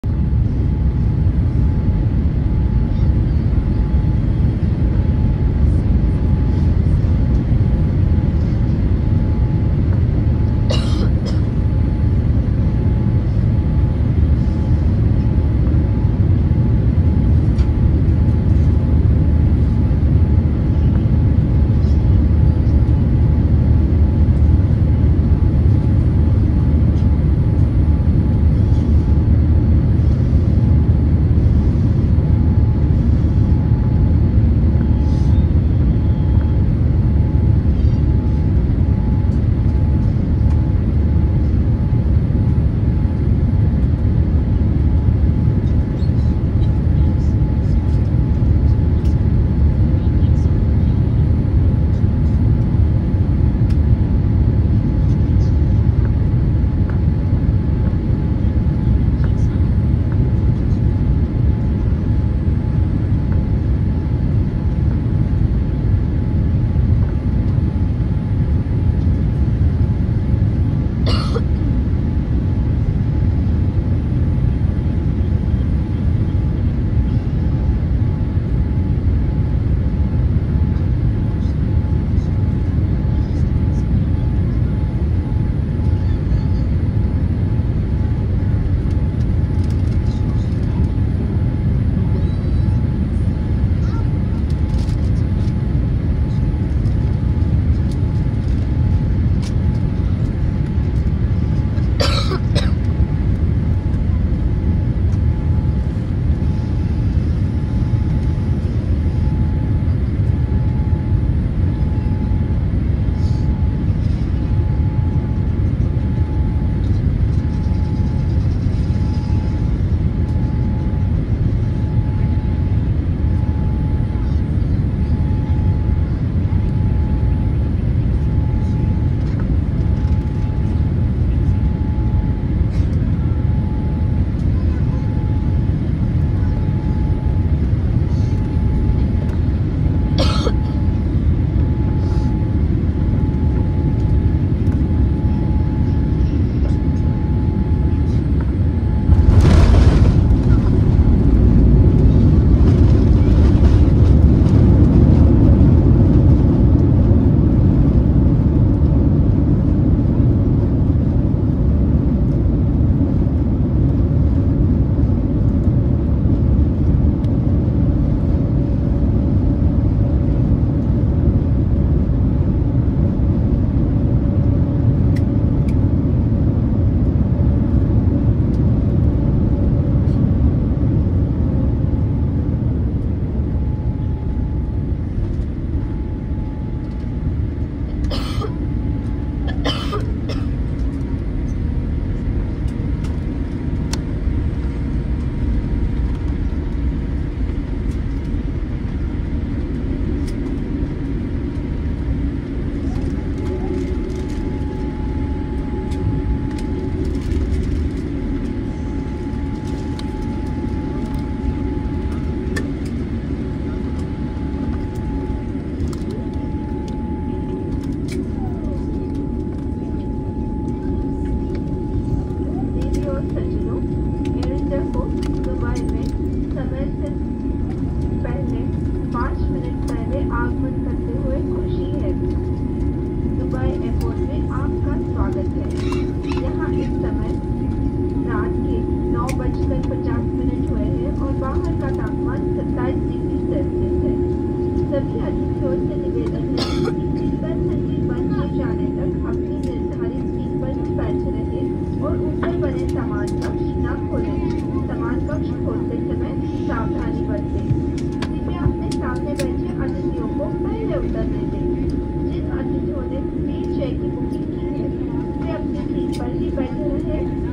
Landing at Dubai International Airport
Air India landing at Dubai International Airport.
Air India Mumbai to Dubai flight.